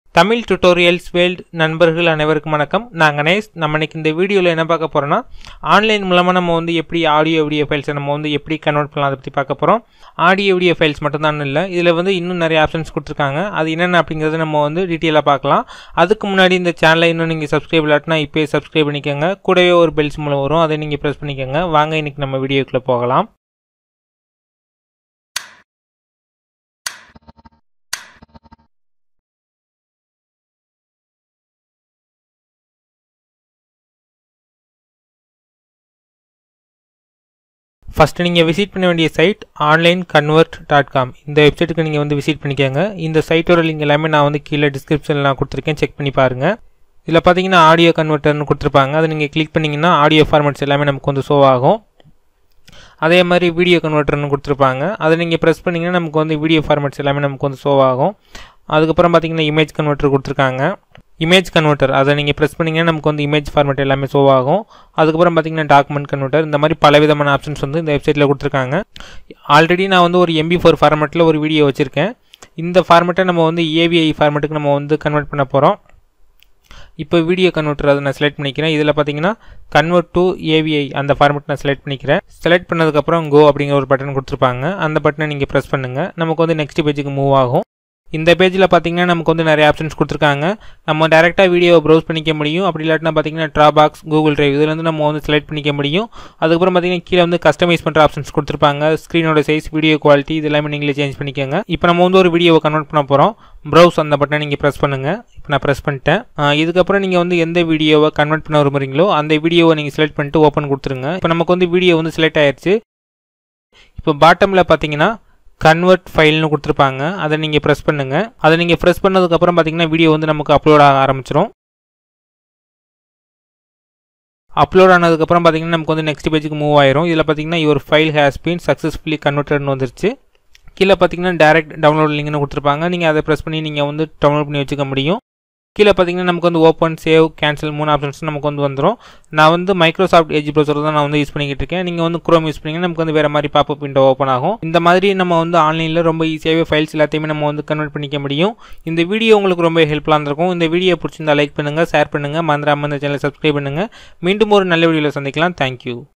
How to Convert Audio videos and More Files in Online Tamil Tutorials World_HD
இந்த வீடியோவில் Online மூலம் Audio மற்றும் வீடியோக்களை Convert செய்வது எப்படி? என்பது பற்றி பார்ப்போம்.